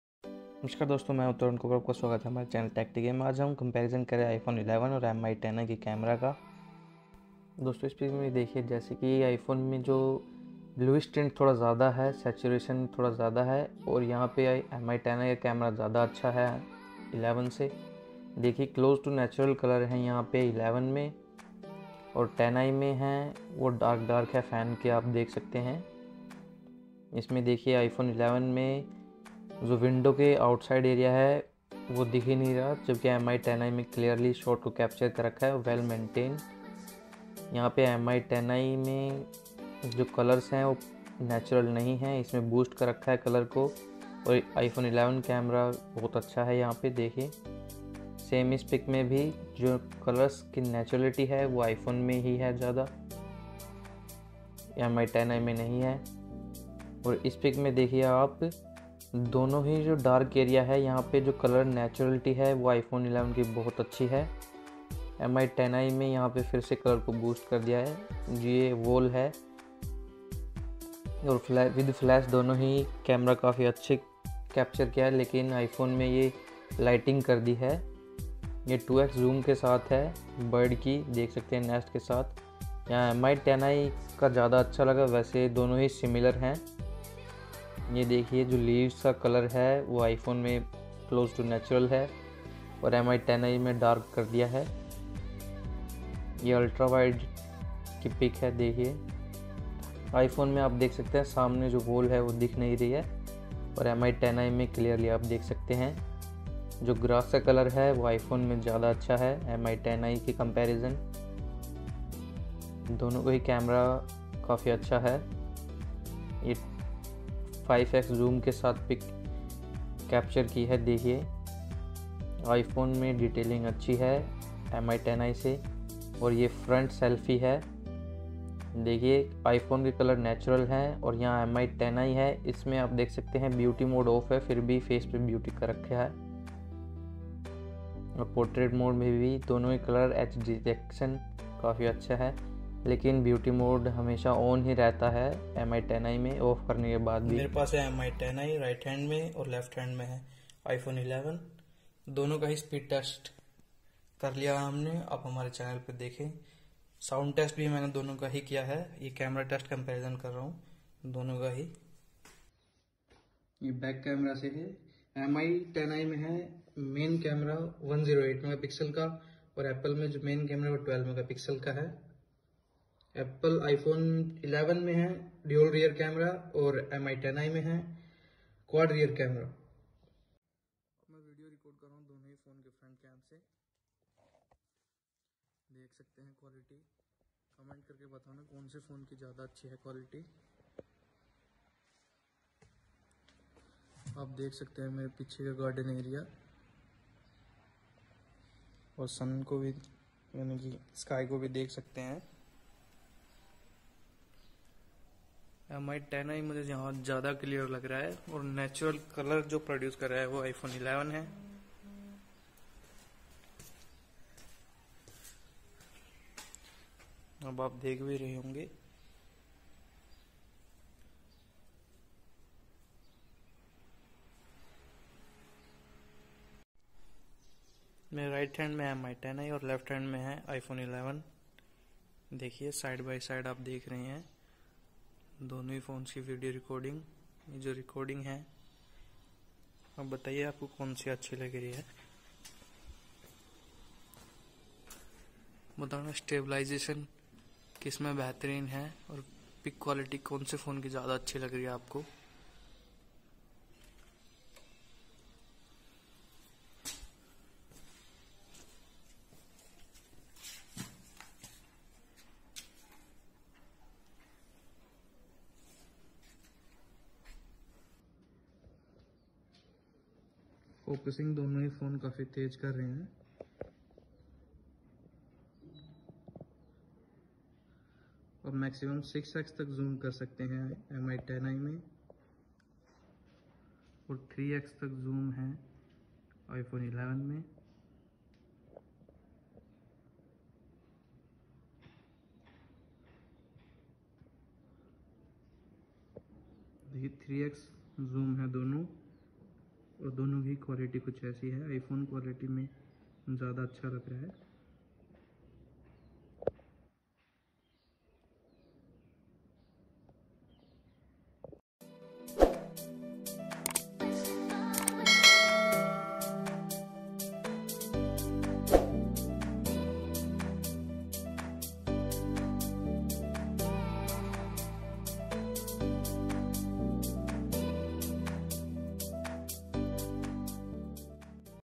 iPhone 11 vs Mi 10i Camera Comparison⚡Shocking🤯Results⚡108MP vs 12MP
Namaskar  Dosto,

Aap sbhi ka swagat ha iss video mein maine Camera Comparison kiya hai Mi 10i vs Apple iPhone 11 device ka chaliye dekhte hain konsa smartphone value for money hai or kiske camera jyada better hai 108MP vs 12MP..